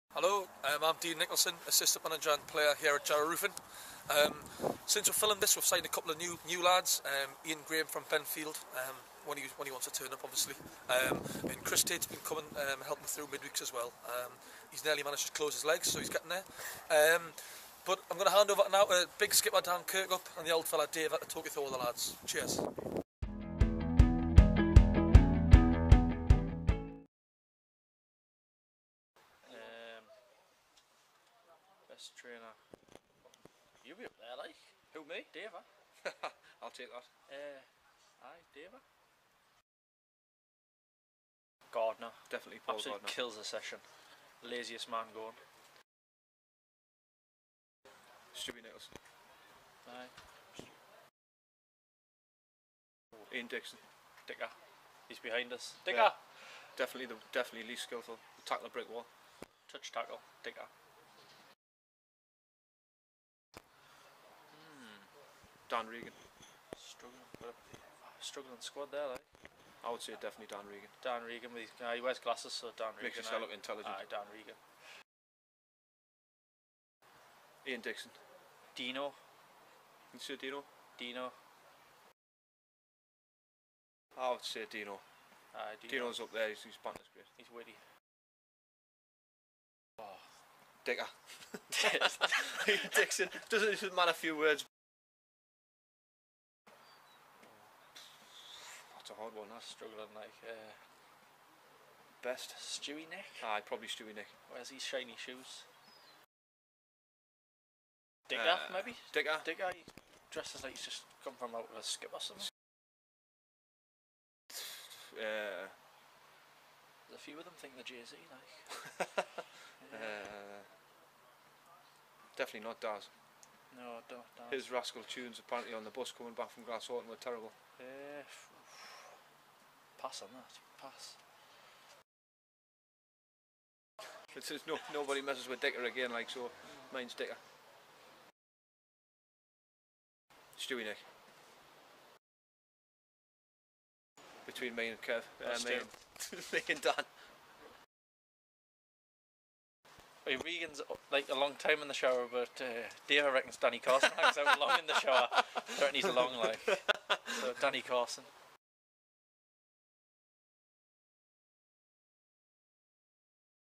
RoofingTV presents - Team Mates
Assistant Manager Dean Nicholson introduces club captain Dan Kirkup and Ian Davison as they talk about the lads in the squad.

Find out who is the fastest player, best dressed, worst banter and er, who spends the longest in the shower.